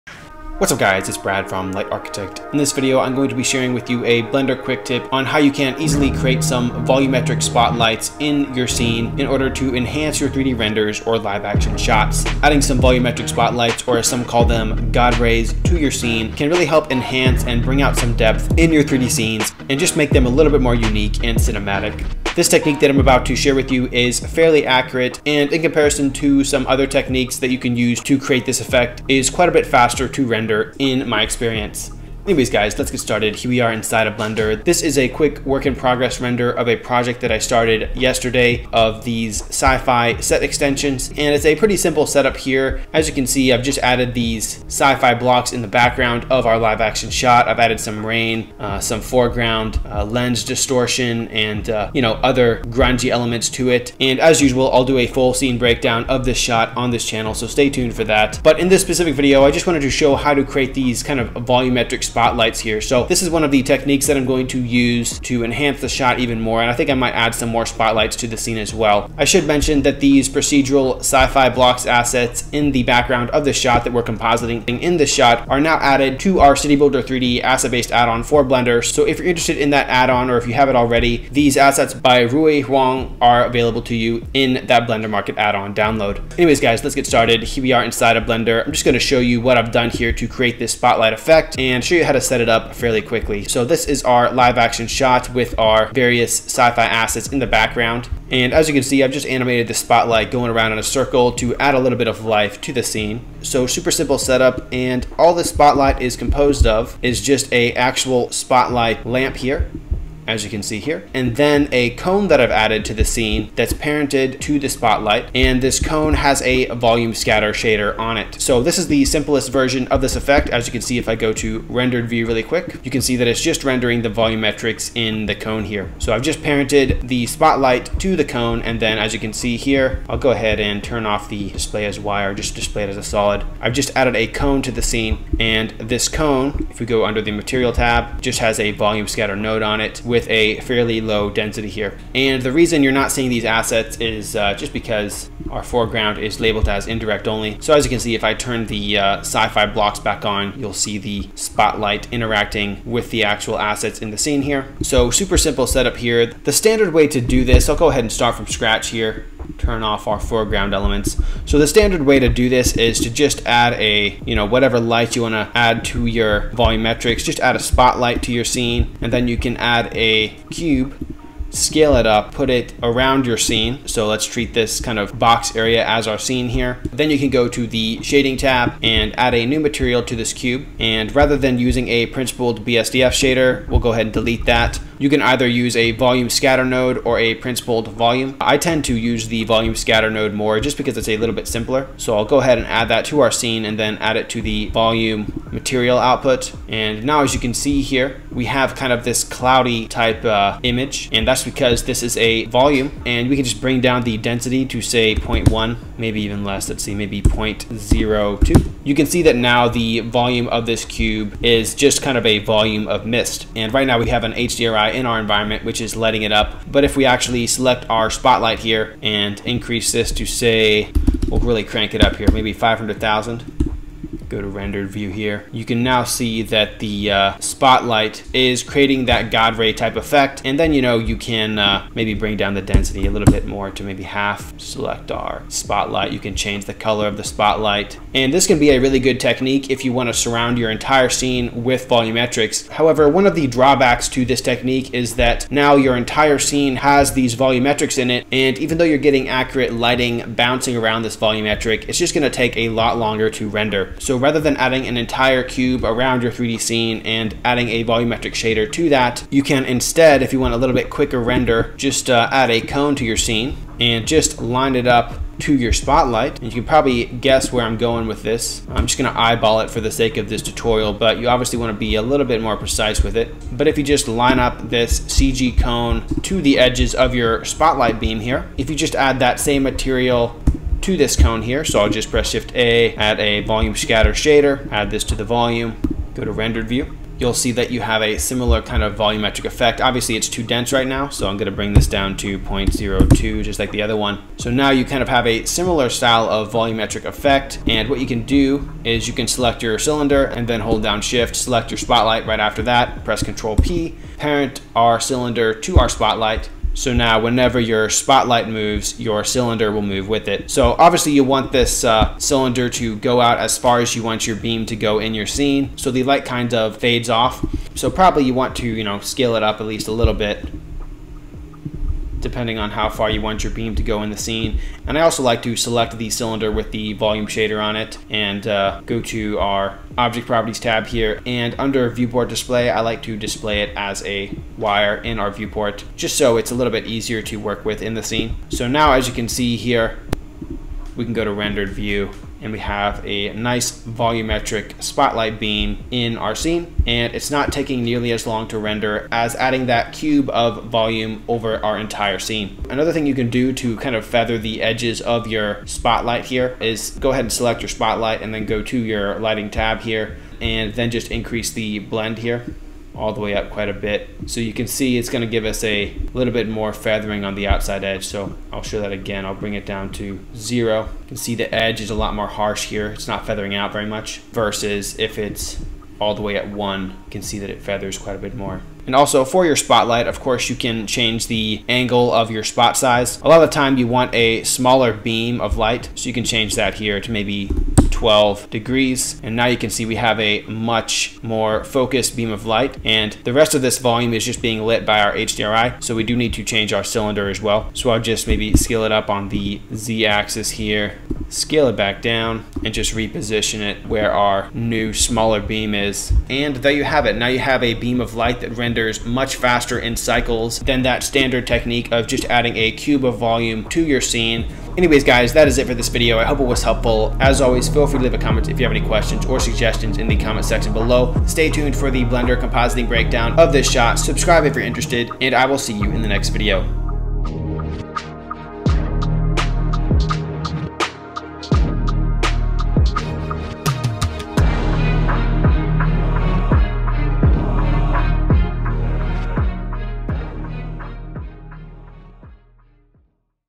Blender Light Beams Tutorial: Lets Make Some God Rays!
Hey Everyone. In this video we share how you can create simple volumetric spotlights in Blender 3d to create a nice light ray effect. This technique is faster to render and more controllable in the vfx compositing process than the standard way of putting a cube of volumetric mist in your 3d scene. This tutorial is for Blender but the concepts can be applied to any 3d program with lights and volumetrics such as Maya, 3dsmax, lightwave, Houdini, cinema4d,  and more. Enjoy and let us know what VFX tips you want to see next on the channel!